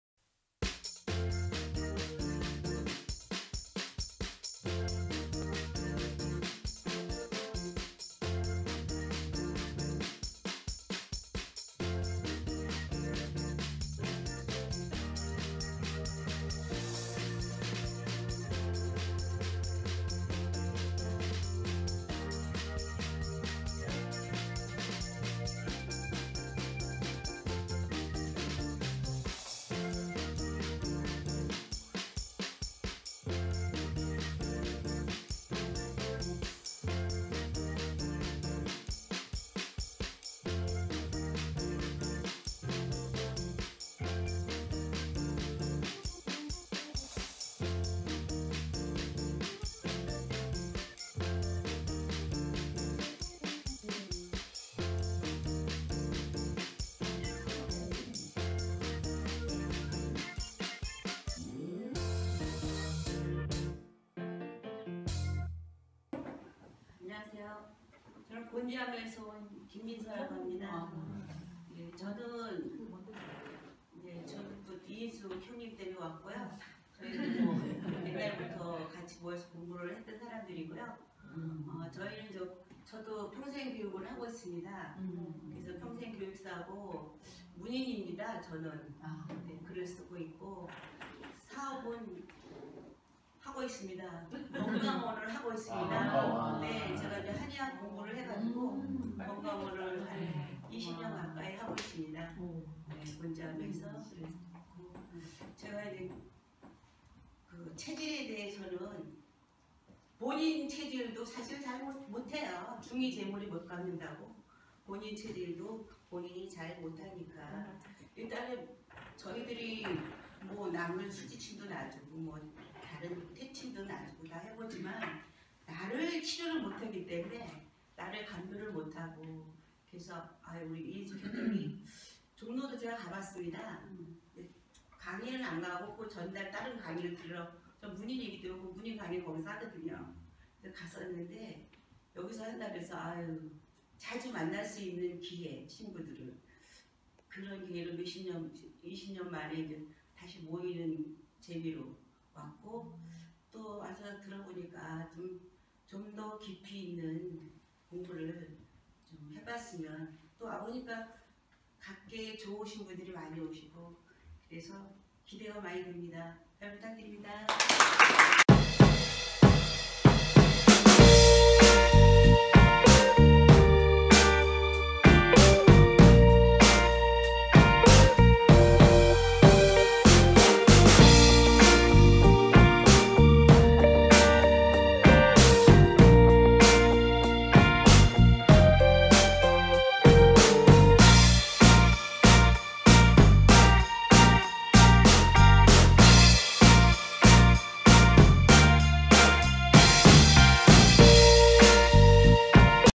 내가 8체질을 공부하는 까닭은?(가천대 1기 김민서)
내가 8체질을 공부하는 까닭은?

(4050건강웰니스 원시인 건강법)
가천대 평생교육원 1기 김민서 선생님

평생교육사이시며 글을 쓰는 문인이시고
한의학의 공부를 바탕으로 20여 년째 건강원도 운영하시는 분이네요.
"중이 제 머리를 못 깎는다."고 말씀은 하시지만,
평생동안 교육을 통해 몸과 마음을 건강하게 살찌우는 방법을 전파하시는 분이네요.
여기에 8체질까지 구비하시면 천하무적이 되겠습니다.